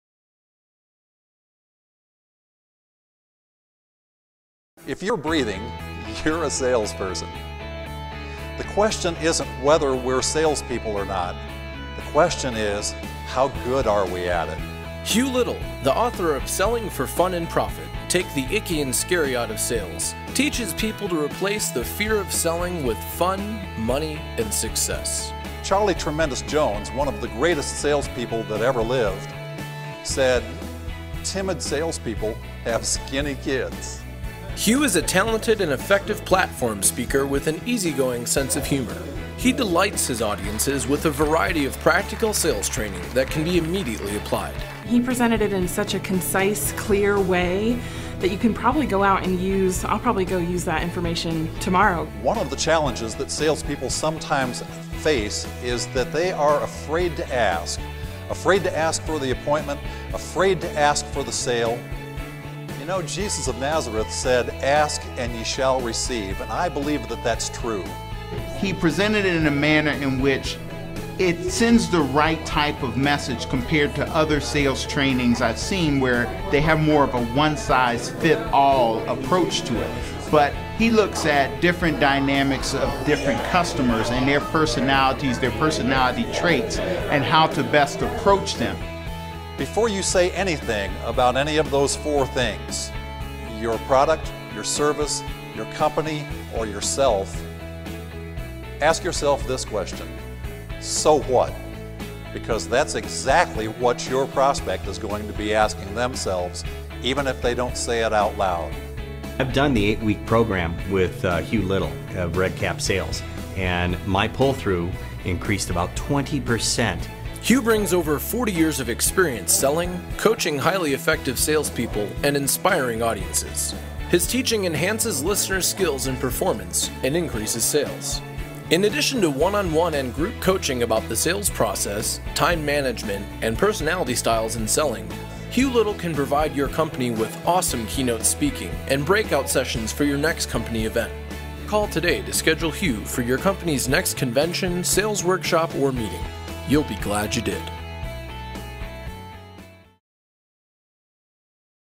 Successful Sales Coaching Strategies
Successful Sales Coaching tips by expert Sales Coach Hugh Liddle, the founder of Red Cap Sales Coaching